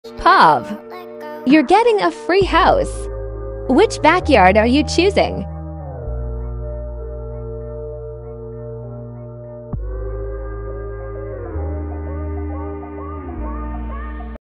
Which backyard are you choosing?
In this YouTube short, we're presenting you with a range of options for your dream backyard. Whether you're looking for a peaceful oasis to relax in, a space for outdoor entertaining, or an area for your kids to play, we've got you covered.

Through a series of quick clips, we'll showcase the different backyard designs and features available, such as pergolas, fire pits, water features, and more. We'll also highlight the different types of landscaping and lighting options that can help enhance the overall ambiance of your backyard.

As you watch the video, take note of the features that stand out to you the most and think about how they could fit into your lifestyle and preferences. With so many options to choose from, you're sure to find the perfect backyard design that suits your needs and personal style.

So sit back, relax, and let us guide you through the process of choosing your dream backyard. By the end of this YouTube short, you'll have a clear idea of what you want and how to bring your vision to life.